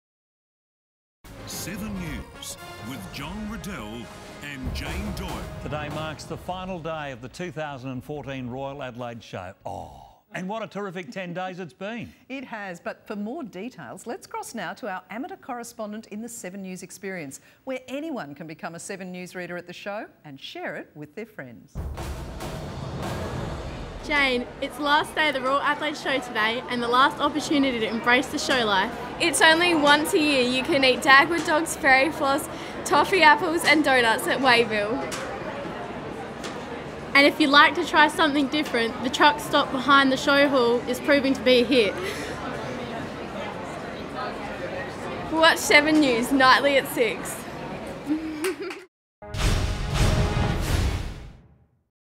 Rachelle and Lane - 7 News Experience
Exclusive report from the Royal Adelaide Show by reporter: Rachelle and Lane